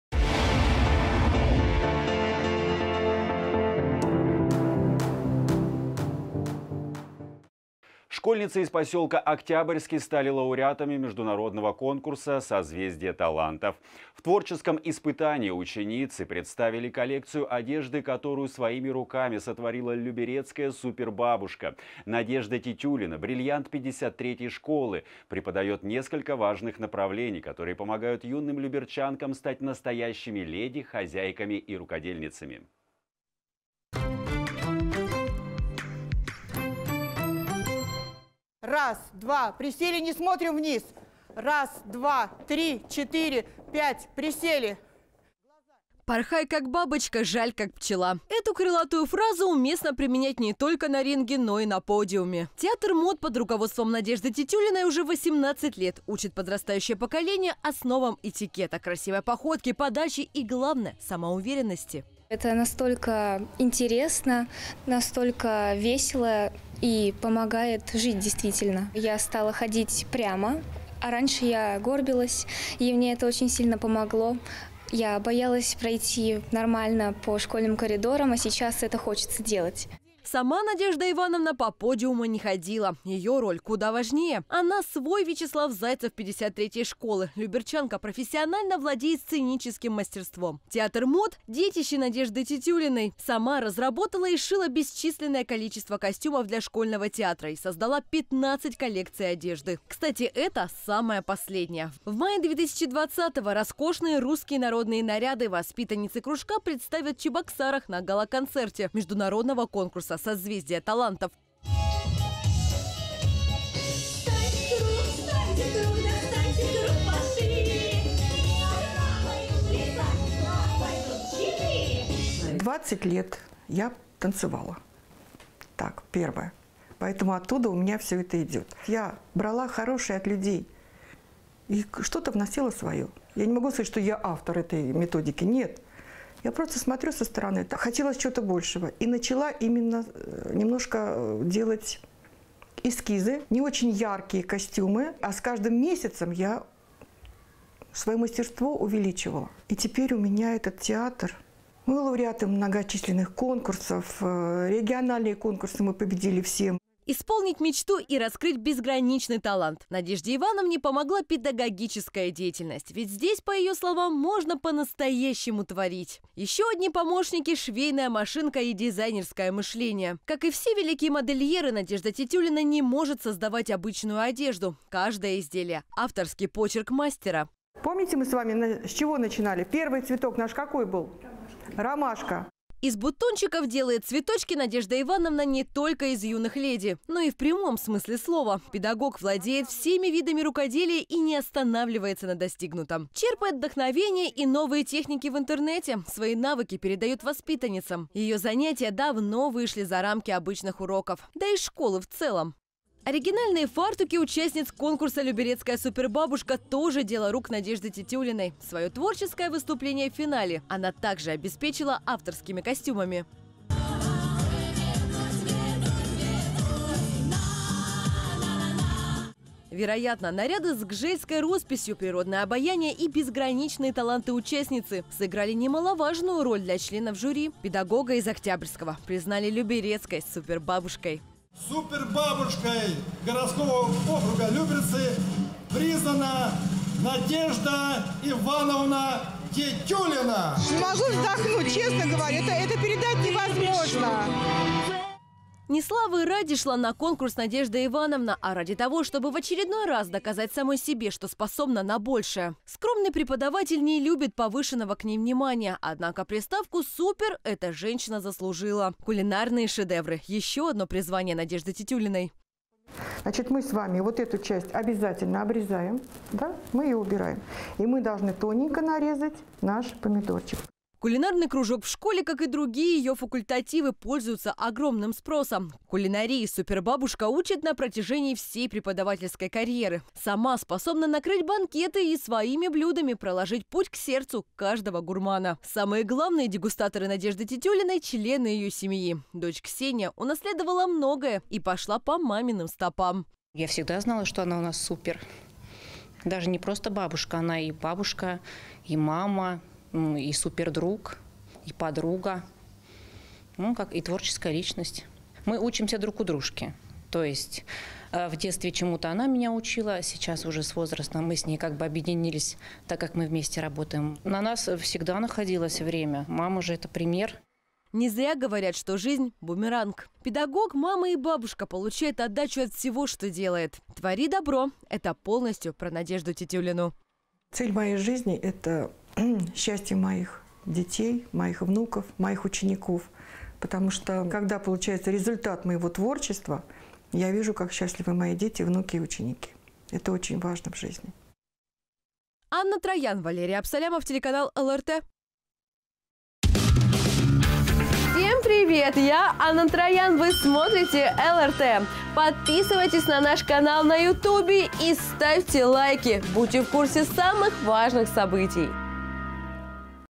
Женщина с приставкой «супер»
Школьницы из поселка Октябрьский стали лауреатами Международного конкурса «Созвездие талантов».  В творческом испытании ученицы представили коллекцию одежды, которую своими руками сотворила «Люберецкая супербабушка». Надежда Тетюлина – «бриллиант» 53-й школы. Преподает несколько важных направлений, которые помогают юным люберчанкам стать настоящими леди, хозяйками и рукодельницами.

Порхай как бабочка, жаль как пчела. Эту крылатую фразу уместно применять не только на ринге, но и на подиуме. «Театр мод» под руководством Надежды Тетюлиной уже 18 лет учит подрастающее поколение основам этикета, красивой походке, подаче, и, главное, самоуверенности.

Валерия Бушуева, воспитанница «Театра мод "Надежда"»:
«Это настолько интересно, настолько весело, и помогает жить, действительно. Я стала ходить прямо, раньше я горбилась. Мне это сильно помогло. Я боялась пройти нормально по школьному коридорам, а сейчас это хочется делать.»

Сама Надежда Ивановна по подиуму не ходила. Ее роль куда важнее. Она свой Вячеслав Зайцев 53-й школы. Люберчанка профессионально владеет сценическим мастерством. Театр мод – детище Надежды Тетюлиной. Сама разработала и сшила бесчисленное количество костюмов для школьного театра и создала 15 коллекций одежды. Кстати, эта – самая последняя. В мае 2020-го роскошные русские народные наряды воспитанницы кружка представят в Чебоксарах на гала-концерте Международного конкурса «Созвездие талантов».

Надежда Тетюлина, учитель технологии школы No 53 г.о. Люберцы:
«20 лет я танцевала. Оттуда у меня это всё идет. Я брала хорошее от людей и что-то вносила свое. Я не могу сказать, что я автор этой методики. Я просто создаю свое. Хотелось чего-то большего. Начала понемногу делать эскизы. Не очень яркие костюмы. С каждым месяцем я свое мастерство увеличивала. И, теперь у меня этот театр. Мы лауреаты многочисленных конкурсов. Региональные конкурсы мы победили все.»

Исполнить мечту и раскрыть безграничный талант Надежде Ивановне помогла педагогическая деятельность. Ведь здесь, по ее словам, можно по-настоящему творить. В этом ей помогла швейная машинка и дизайнерское мышление. Как и все великие модельеры, Надежда Тетюлина не может создавать обычную одежду. Каждое изделие – «авторский почерк» мастера.

Из бутончиков делает цветочки Надежда Ивановна не только из юных леди, но и в прямом смысле слова. Педагог владеет всеми видами рукоделия и не останавливается на достигнутом. Черпает вдохновение и новые техники в интернете. Свои навыки передает воспитанницам – ее занятия давно вышли за рамки обычных уроков, да, и школы в целом.

Оригинальные фартуки участниц конкурса «Люберецкая супербабушка» тоже дело рук Надежды Тетюлиной. Свое творческое выступление в финале она также обеспечила авторскими костюмами.

Вероятно, наряды с гжельской росписью, природное обаяние и безграничные таланты участницы сыграли немаловажную роль для членов жюри. Педагога из Октябрьского признали «люберецкой супербабушкой».

Ни славы ради шла на конкурс Надежда Ивановна. А ради того, чтобы в очередной раз доказать самой себе, что способна на большее. Скромный преподаватель не любит повышенного к ней внимания. Однако, приставку супер эта женщина заслужила. Кулинарные шедевры – еще одно призвание Надежды Тетюлиной.

Кулинарный кружок в школе, как и другие ее факультативы, пользуются огромным спросом. Кулинарии супербабушка учит на протяжении всей преподавательской карьеры. Сама способна накрыть банкеты и своими блюдами проложить путь к сердцу каждого гурмана. Самые главные дегустаторы Надежды Тетюлиной – члены ее семьи. Дочь Ксения унаследовала многое и пошла по маминым стопам.

Ксения Гесть, дочь Надежды Тетюлиной:
«Я всегда знала, что она супер. Она не просто бабушка, и мама, и супер друг, и подруга, и творческая личность. Мы учимся друг у друга. В детстве чему-то она меня учила. Сейчас уже с возрастом мы объединились, так как мы вместе работаем. На нас всегда находилось время. Мама – это пример.»

Не зря говорят, что жизнь бумеранг. Педагог, мама и бабушка получает отдачу от всего, что делает. «Твори добро» – это полностью про Надежду Тетюлину.

Надежда Тетюлина, учитель технологии школы No53 г.о. Люберцы:
«Цель моей жизни – это счастье моих детей, моих учеников, потому что, когда получается результат моего творчества, я вижу, как счастливы мои дети, мои внуки, мои ученики. Это очень важно в жизни.»